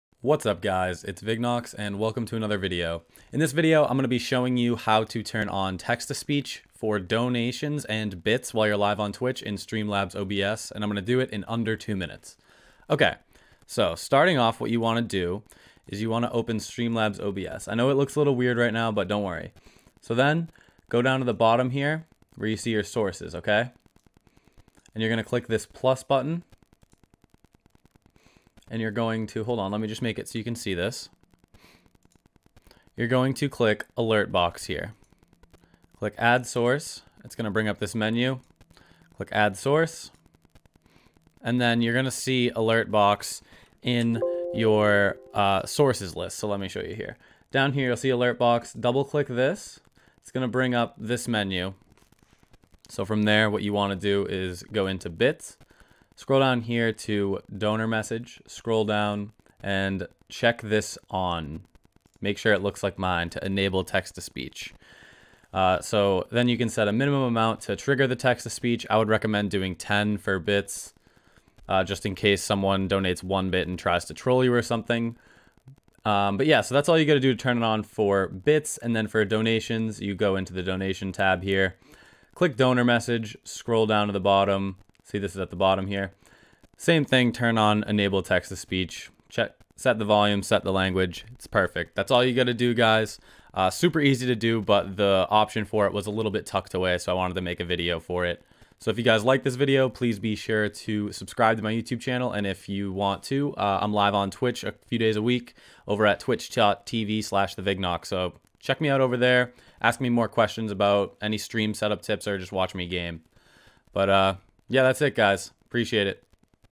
How to Add Text-to-Speech Donations to Your Twitch Stream in Under 2 Minutes (Streamlabs OBS)
Hi everyone! I wanted to make a quick video on how to add text-to-speech to your donation alerts in Streamlabs OBS.